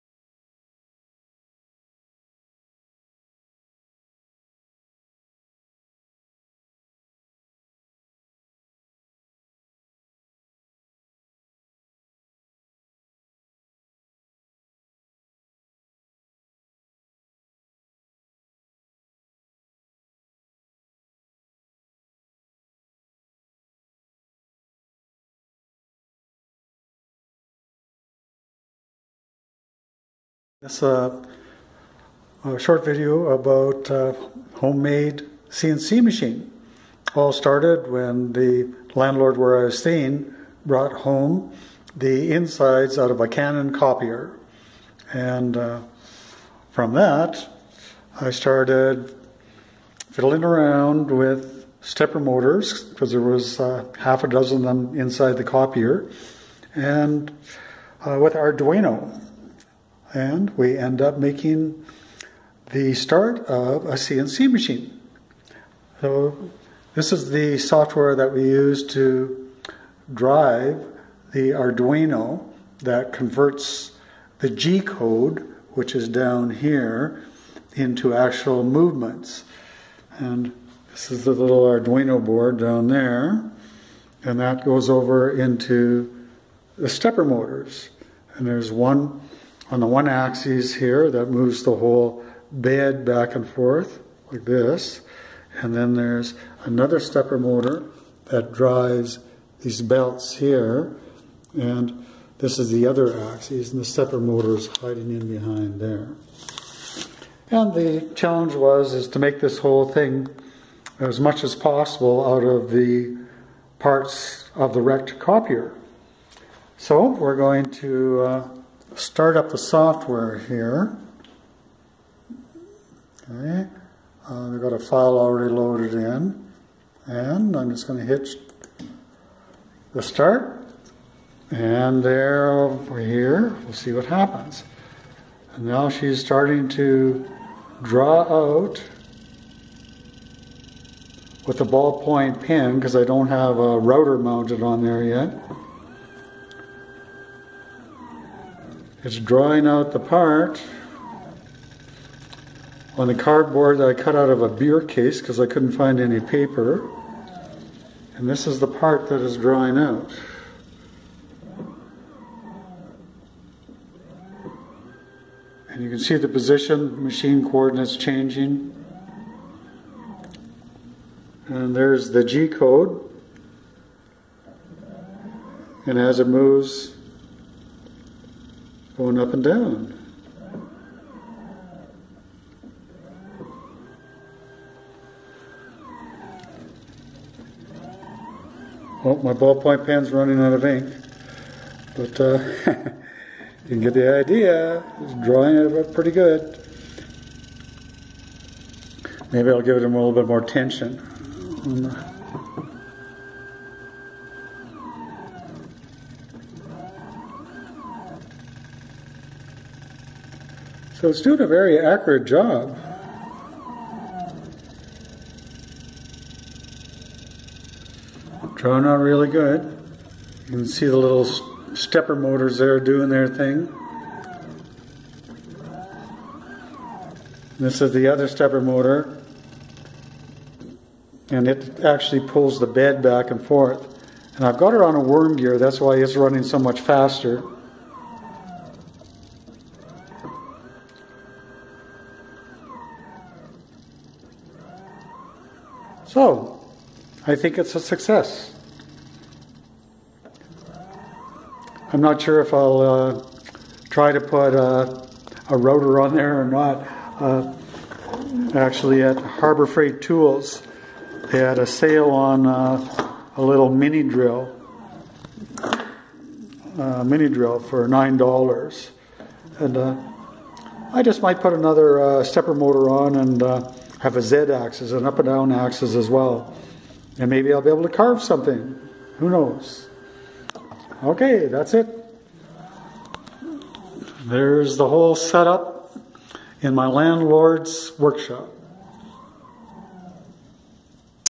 CNC build from a Canon copier parts
The owner of the house where I am staying in Las Vegas brought home the insides of a Canon copier from his work for me to play with.  I always wanted to figure out how a CNC machine worked, so I built one!